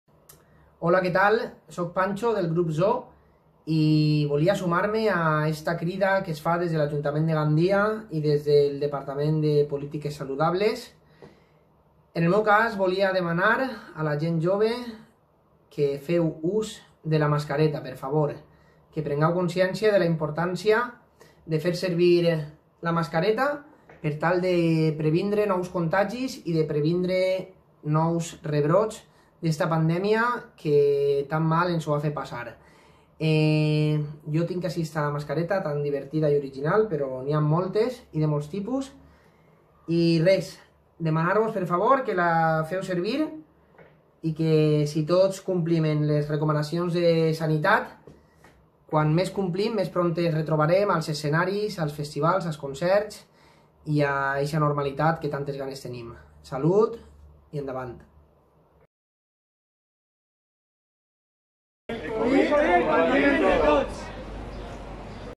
Ajuda'ns i ajuda't, posa't la mascareta. Ajuntament de Gandia sanitat, i Zoo "Pancho".
Recomanacions.  Ajuda'ns i ajuda't a tu mateix, segueix les recomanacions, posa't la mascareta, respecta el distanciament físic i llavat les mans.